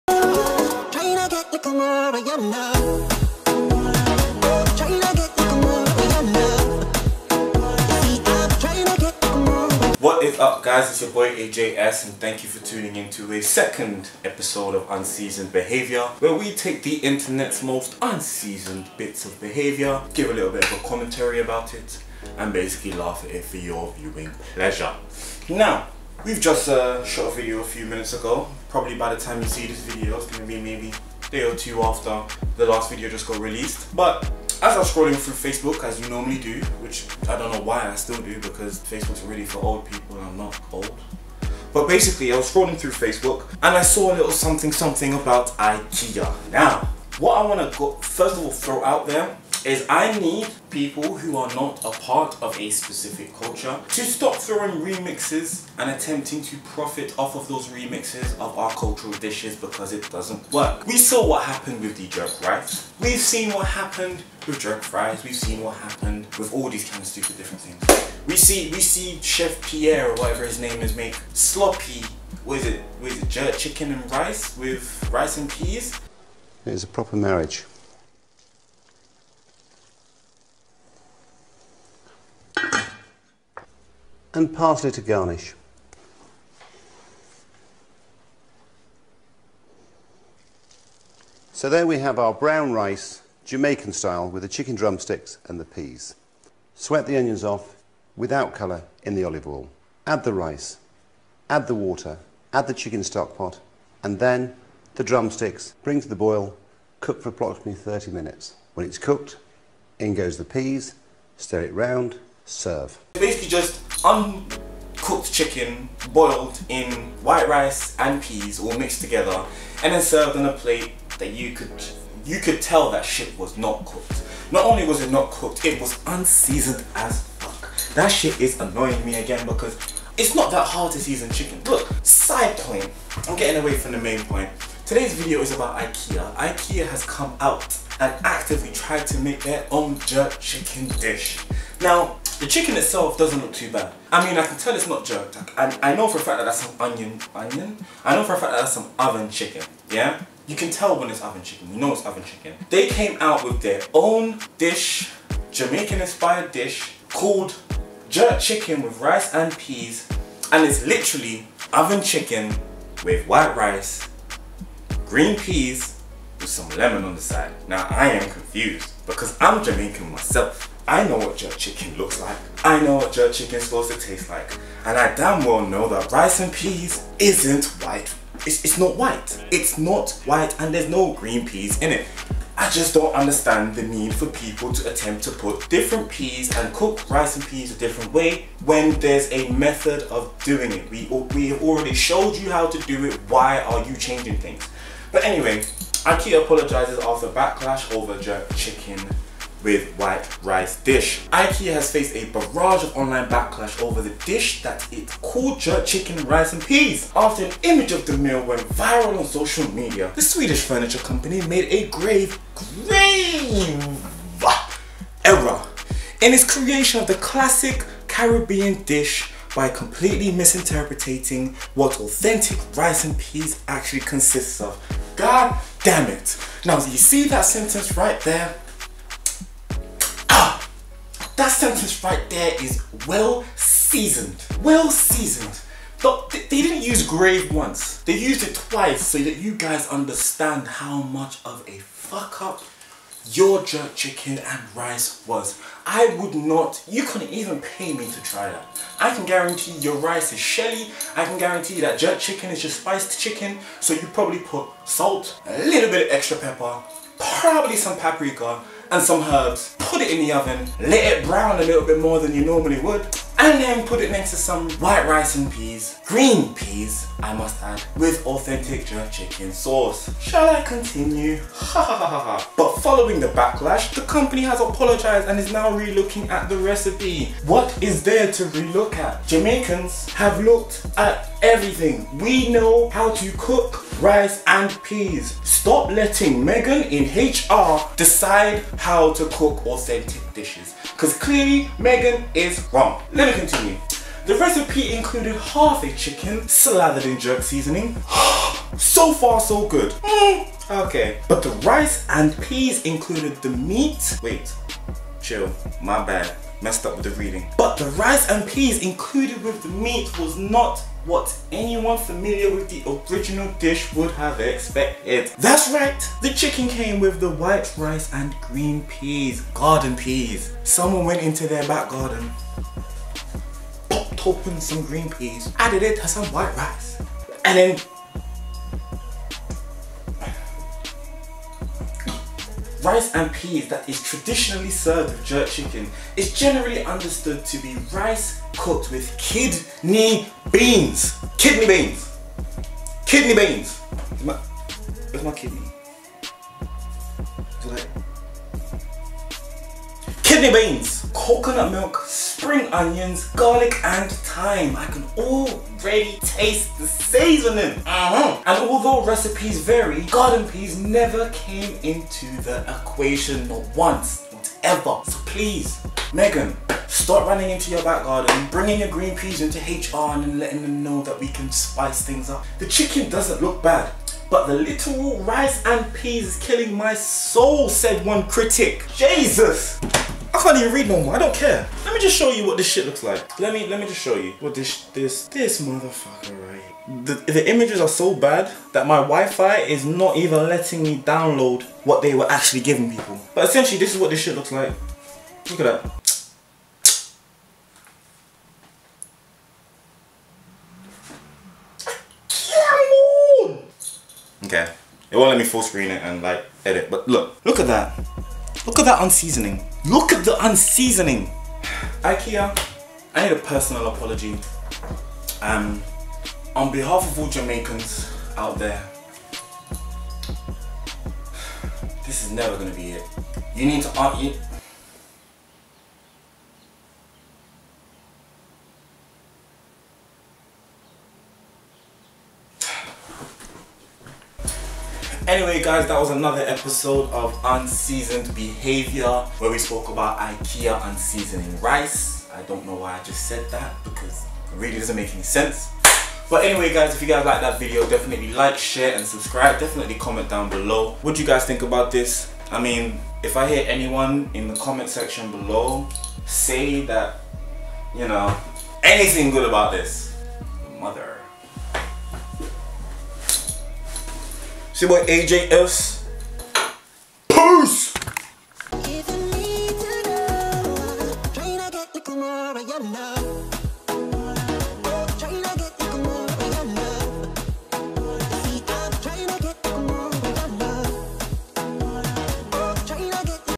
IKEA apologies for jerk chicken recipe
IKEA Apologies for Jerk chicken Dish | Unseasoned Behavior Episode 1

SMASH 100 likes for more public videos